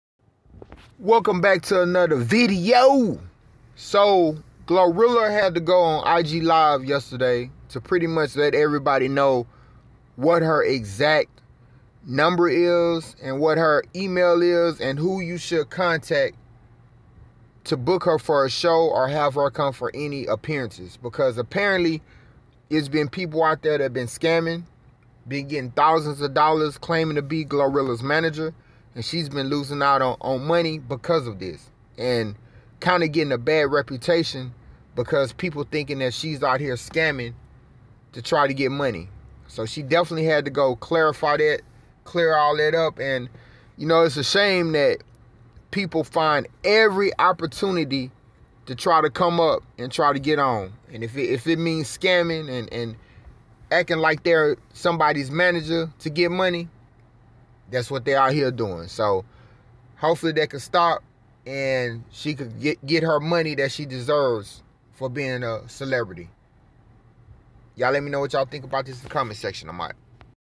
Glorilla Been Getting Scammed Out Of Money For Shows
The scammers have hit glorilla and tried to pretend to be her management team